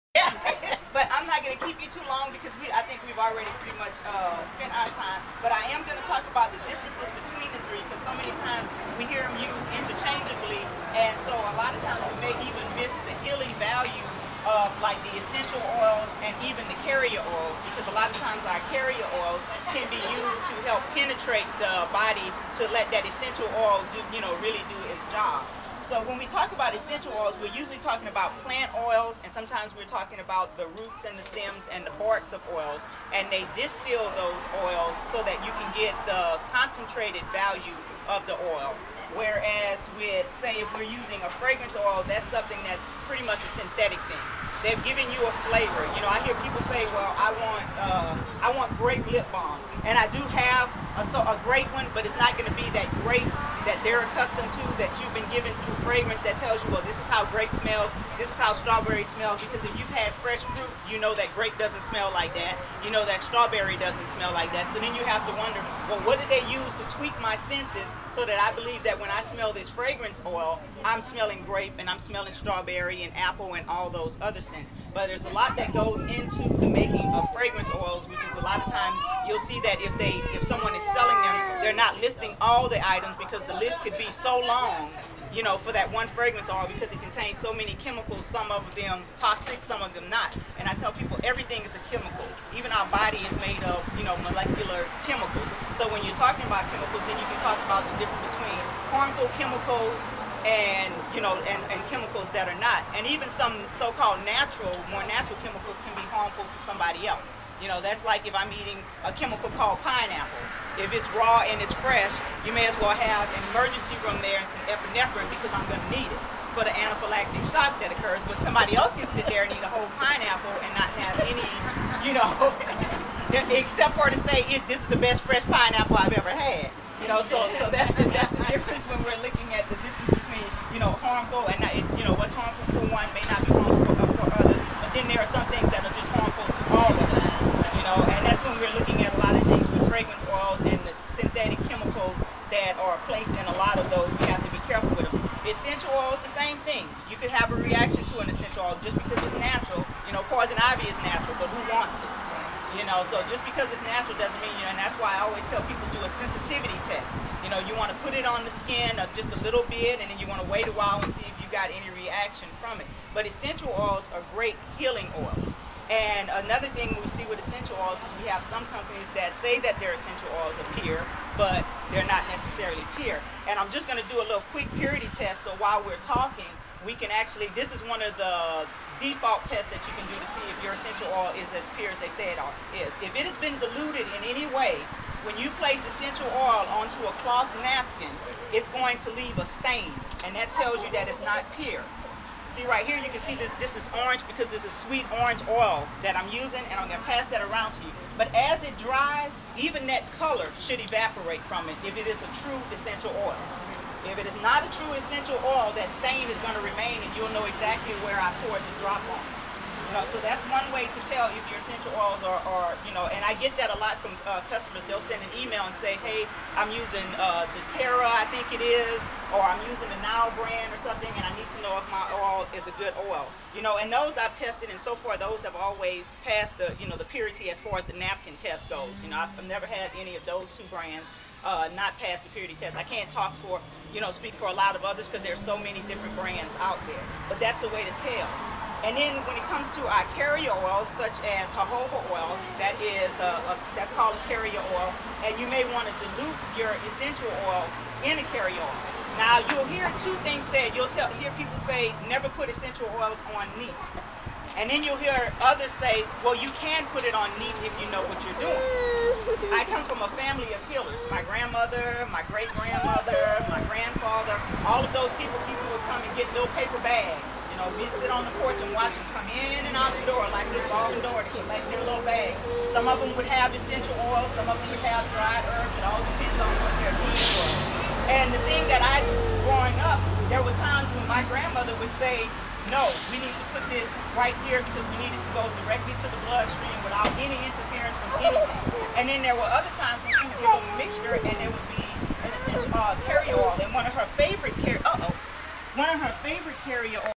Carrier, Fragrance and Essential Oils Nandi's Naturals YouTube2
Nandi's Naturals owner discusses Carrier, Fragrance and Essential Oils. She is a proud vendor at the Blissful Birth & Baby Virtual Expo.
Visit her website for beautiful natural body and hair products that won't disappoint you!